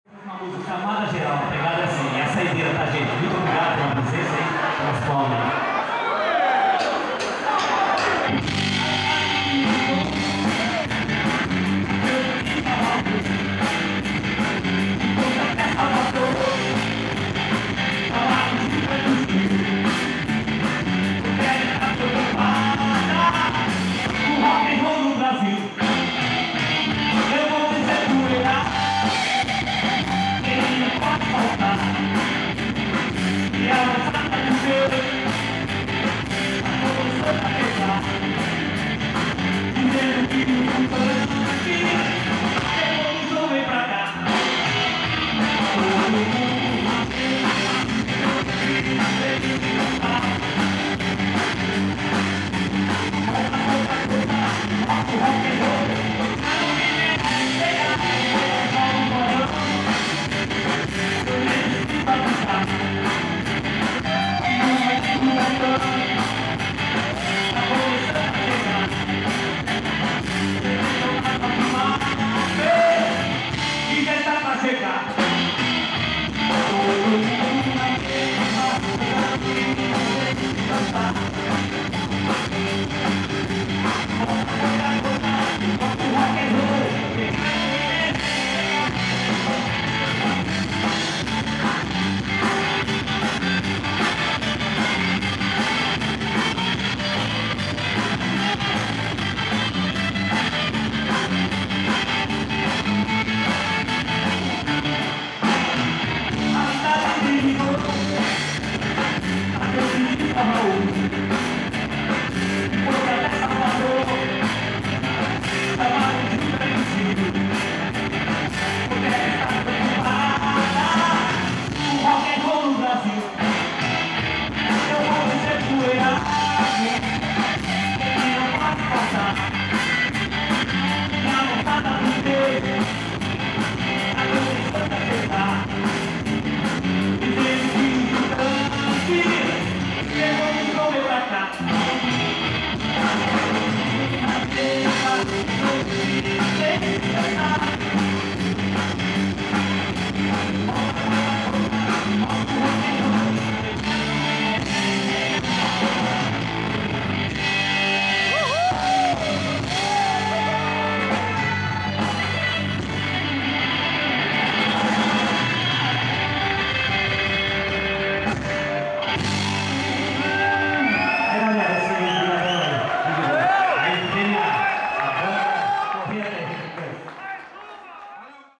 chamada geral
A volta da Banda Talismã Rock Band no Park Rock City em Sapiranga-RS com Pedrinho voz e violão,  Paulo Gordo bateria, Marquinhos guitarra e vocal, e Luis Mendes baixo e vocal no dia 14 de julho de 2012.